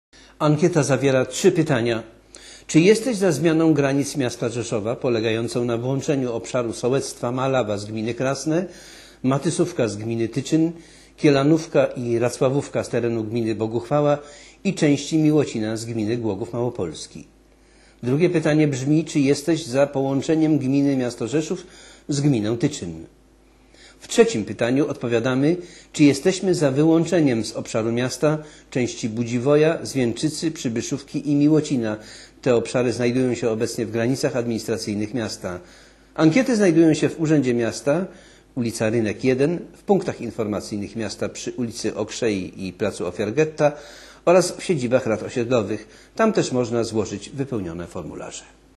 Ankieta - 3 pytania.
Konsultacja wśród mieszkańców Rzeszowa. Mają udzielić odpowiedzi na trzy pytania związane z granicami miasta